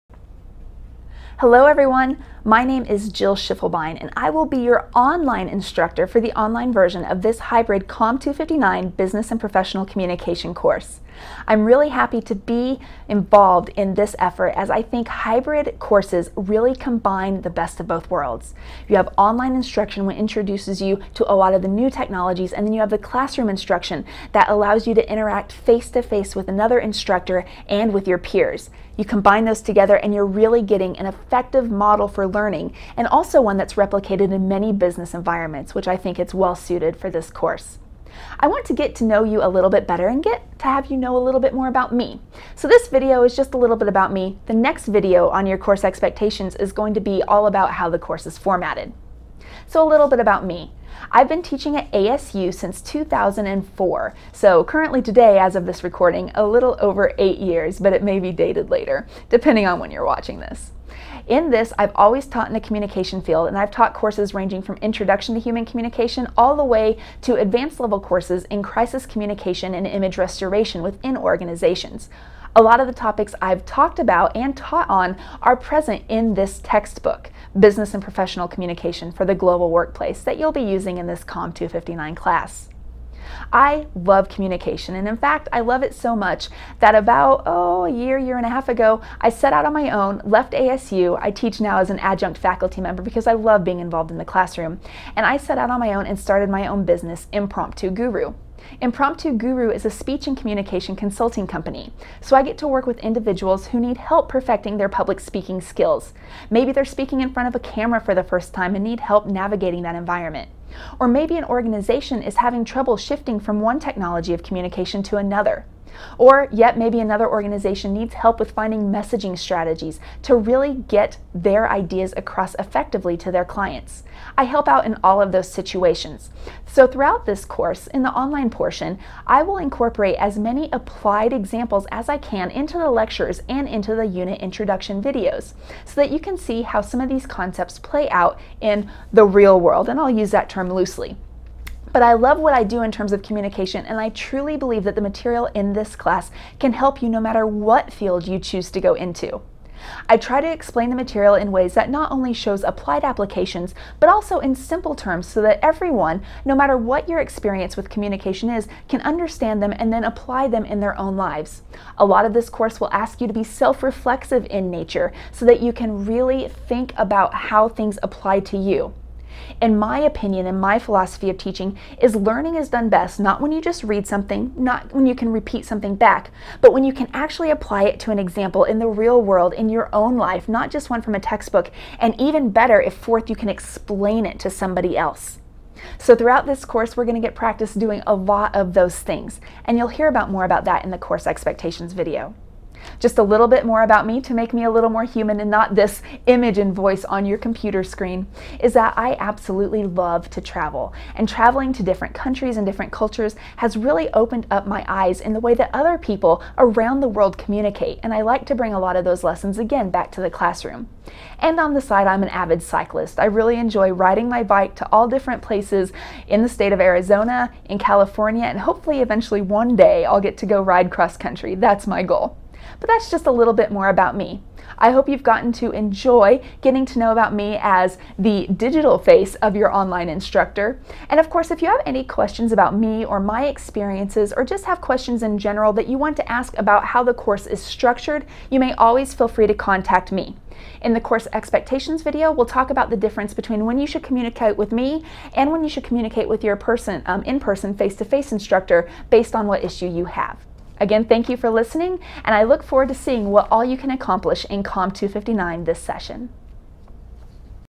COM 259 - Meet your Instructor
Jill Schiefelbein has been an instructor in the Hugh Downs School of Communication at Arizona State University since 2004. In this video she introduces herself as the online instructor of the course.